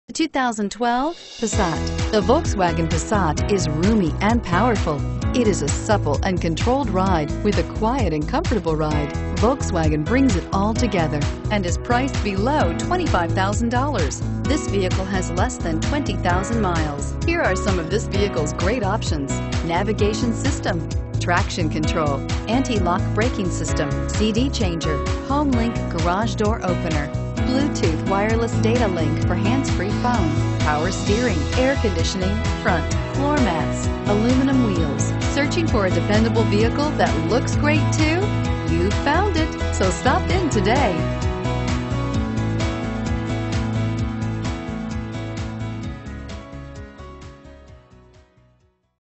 2012 Volkswagen Passat Irvine Orange County PA2703A
Fladeboe VW
20 Auto Center Dr

You'll love this 2012 Volkswagen Passat. This is a car you'll want to take home. With 19812 miles, it features Automatic transmission and an exterior color of .